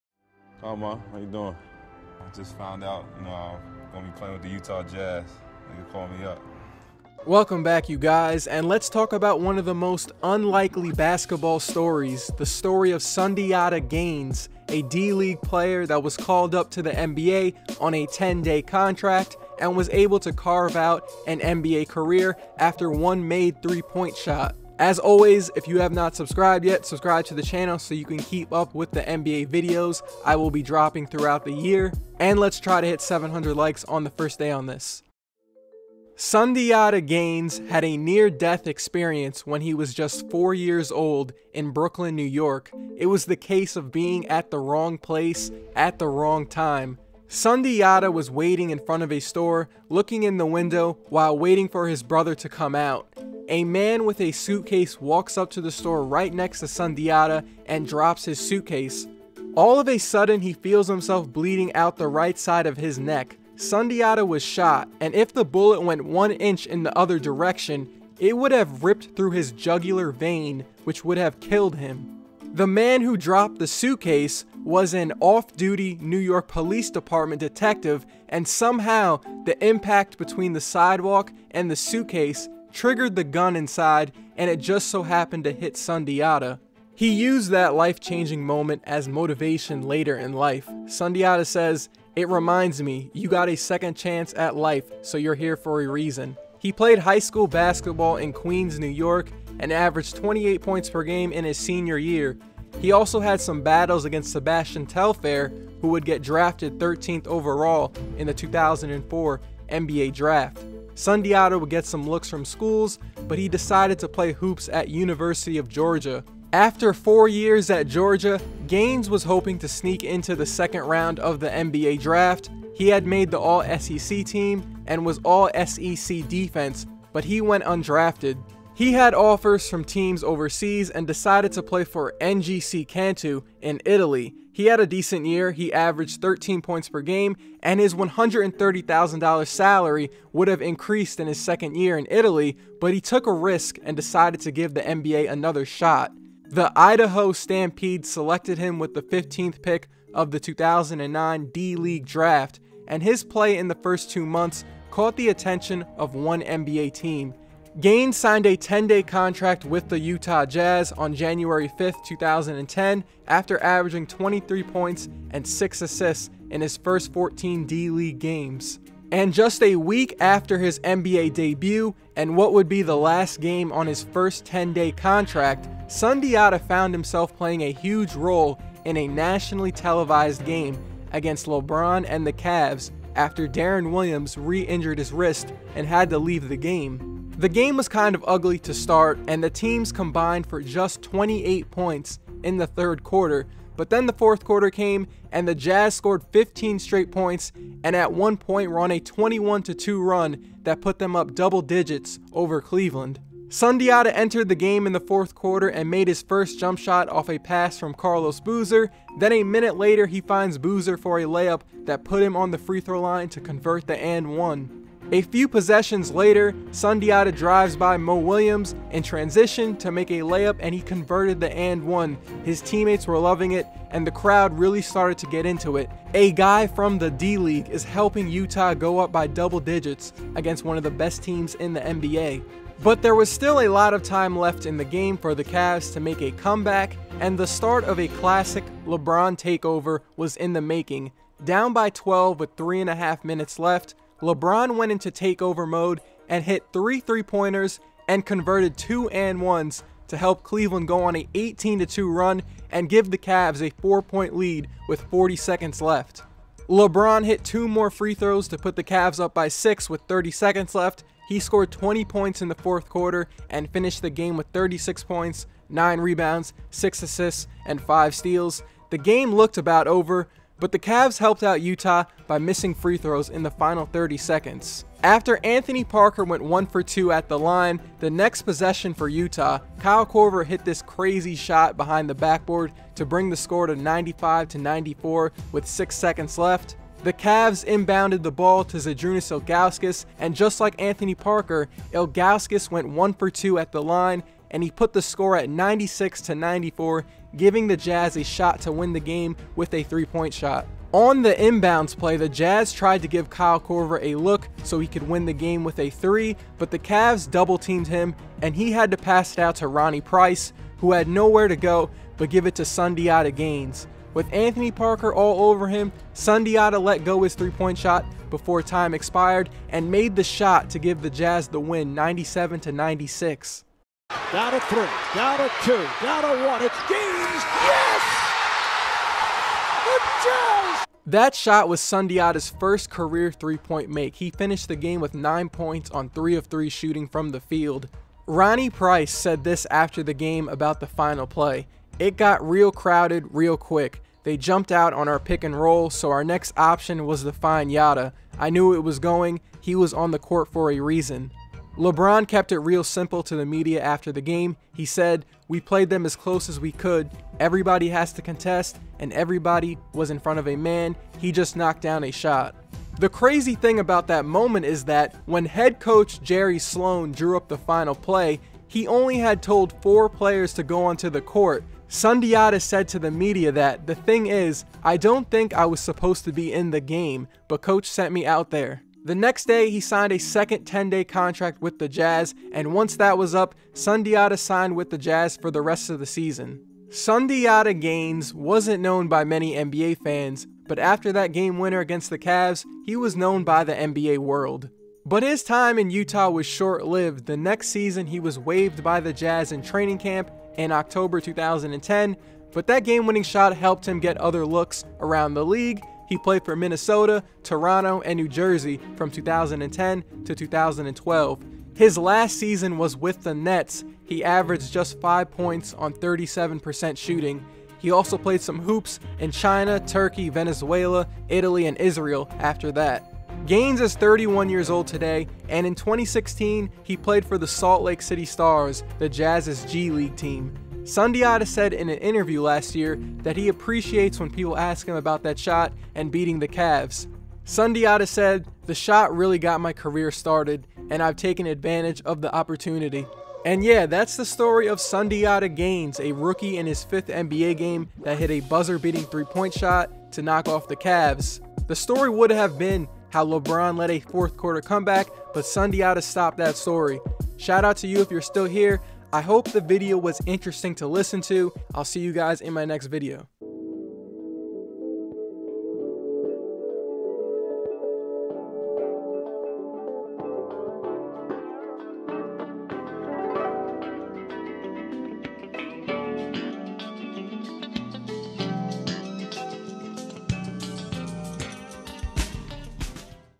Meet the NBA Player That Surprised LeBron’s Cavs in 2010
Meet the NBA Player that Surprised LeBron James and the Cavaliers, NBA with a game winner - a video about the NBA basketball story of Sundiata Gaines, a D League player who got called up by the Utah Jazz. He made one of the biggest jumpers of his life to stun LeBron James and the Cleveland Cavaliers.